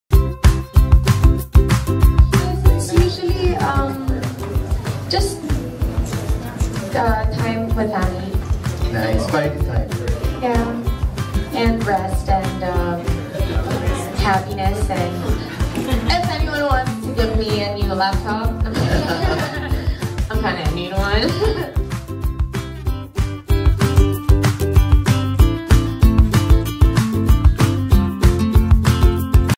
Jasmine's Wishlist
Jasmine Curtis-Smith enumerating her whilst for this holiday.

How about you, whats your Wishlist?